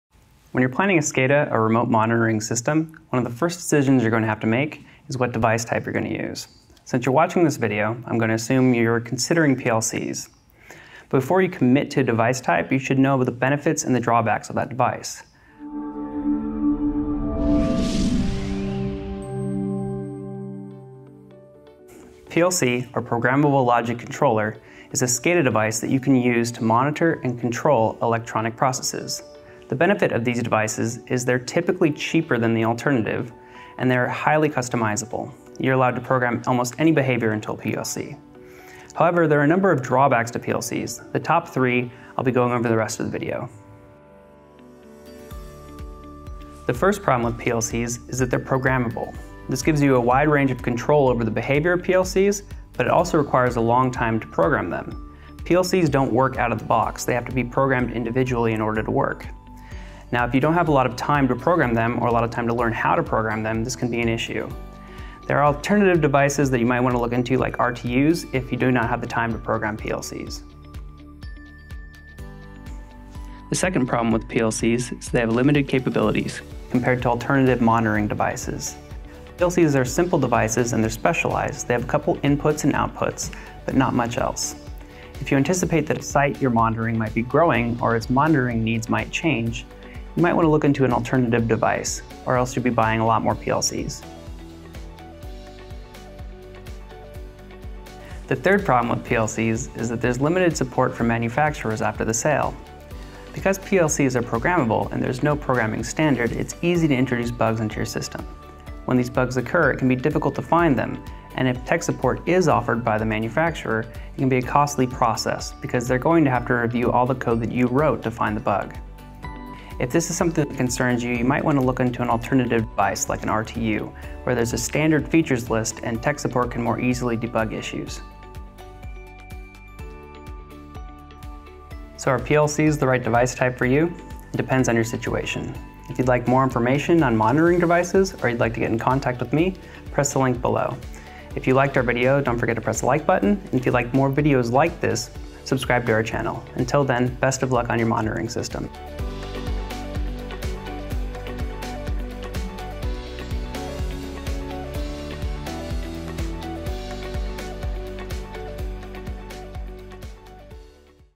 Top 3 Problems with PLCs
PLCs are the backbone of many SCADA systems. They're highly customizable, but that freedom comes with some significant potential problems that you need to know about. There are some PLC alternatives you can consider that have different pros and cons.